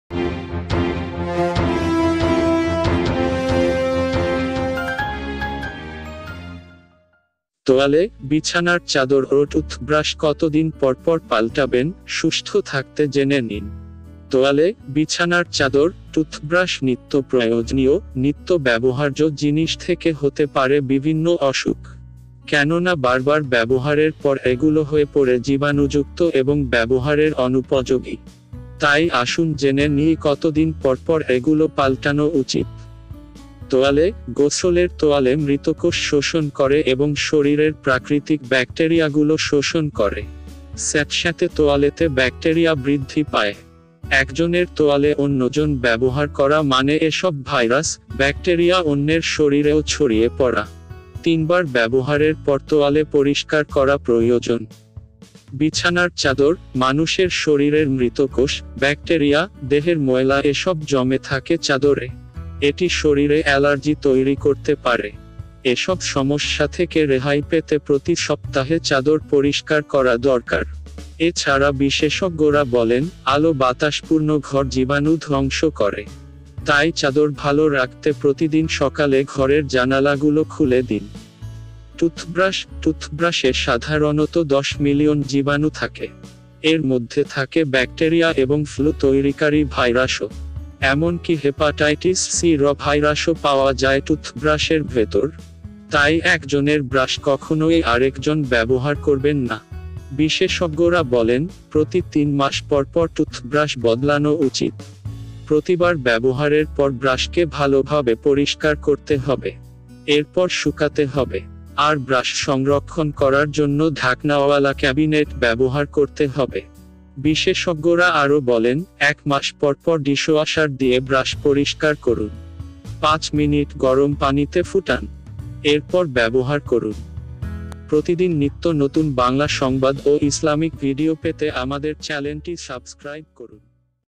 সুস্থ্য থাকতে জেনে নিন !!তোয়ালে, বিছানার চাদর ও টুথব্রাশ কতদিন পরপর পাল্টাবেন
তোয়ালে, বিছানার চাদর, টুথব্রাশ- নিত্যপ্রয়োজনীয়, নিত্য ব্যবহার্য জিনিস থেকে হতে পারে বিভিন্ন অসুখ। কেননা বারবার ব্যবহারের পর এগুলো হয়ে পড়ে জীবাণুযুক্ত এবং ব্যবহারের অনুপযোগী।
তাই আসুন জেনে নিই কতদিন পরপর এগুলো পাল্টানো উচিত।